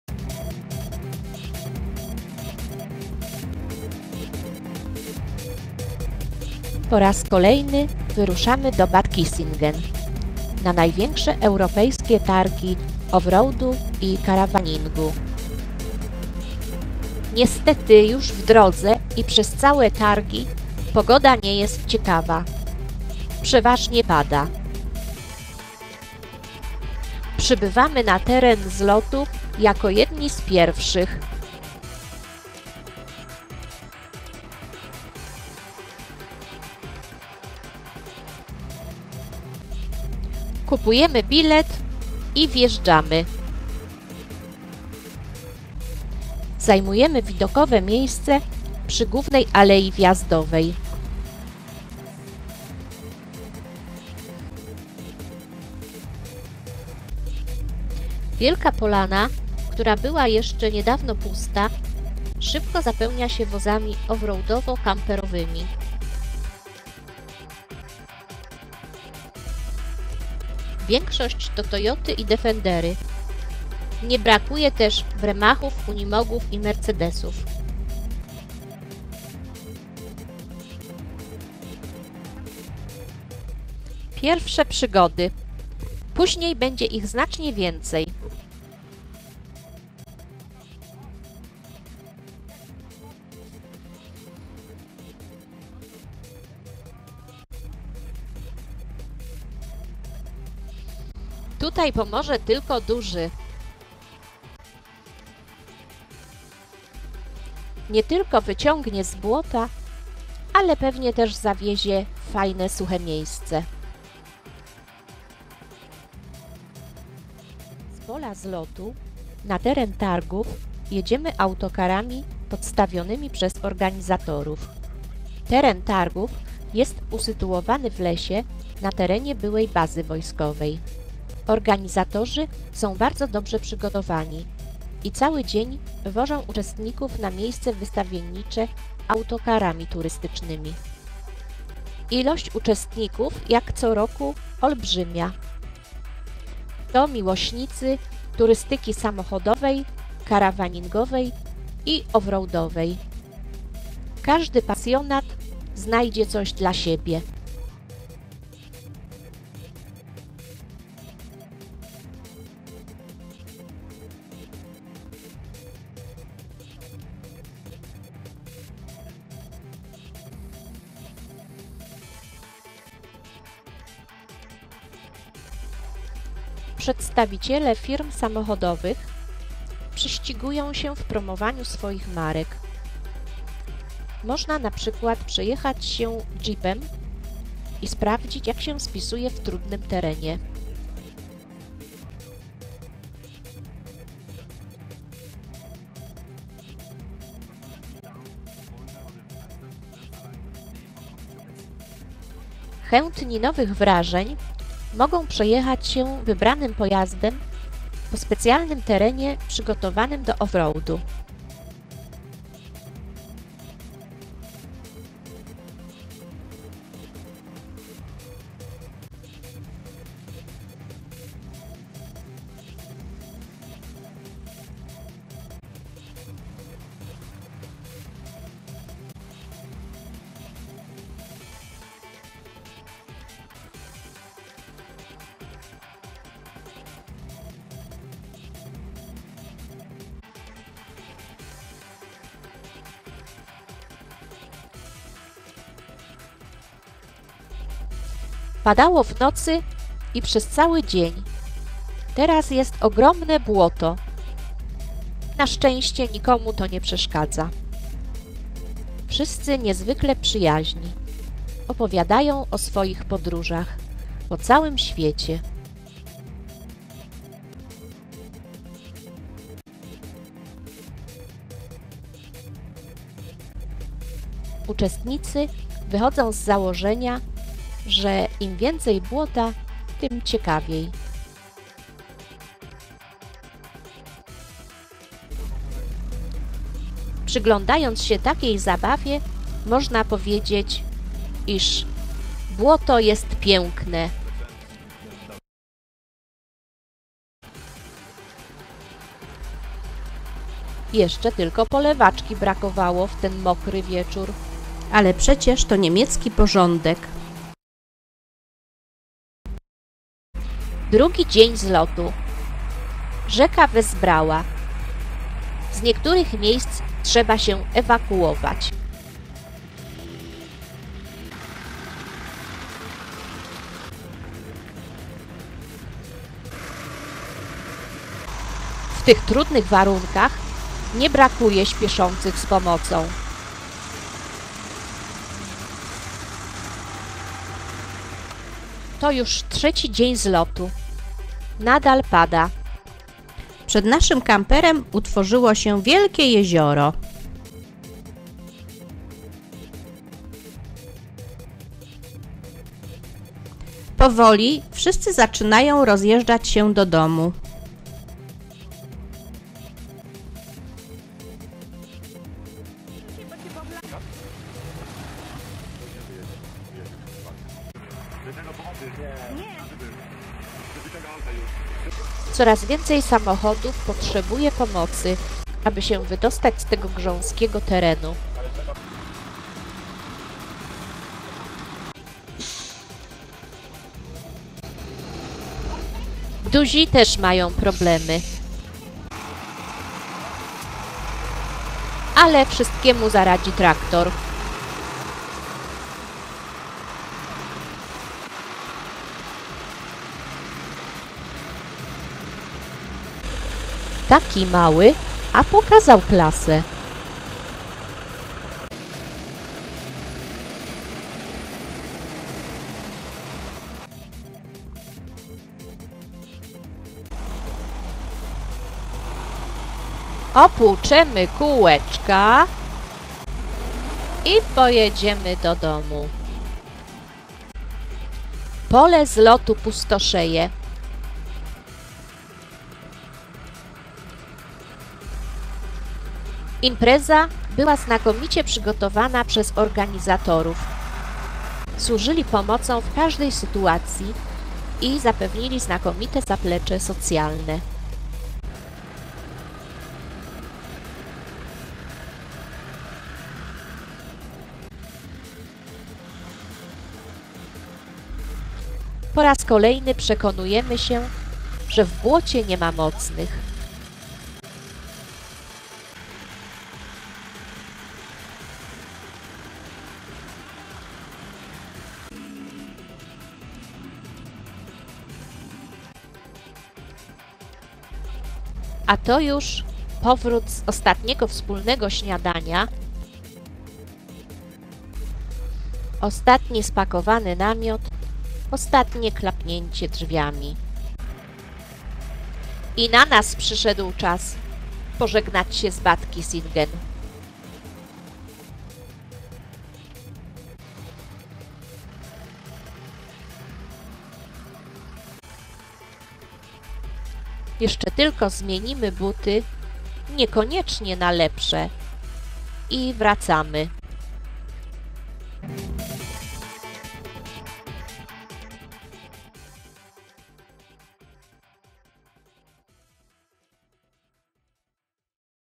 Abenteuer & Allrad Messe in Bad Kissingen
W Bad Kissingen  co roku odbywa się największy w Europie pokaz i targi  off roadowe. Warto jechać na te targi W roku 2015 planowane są od 04.06 do 07.06. My byliśmy dwa razy i za każdym razem było fajnie. No może w 2013r za mocno padało, ale i tak polecam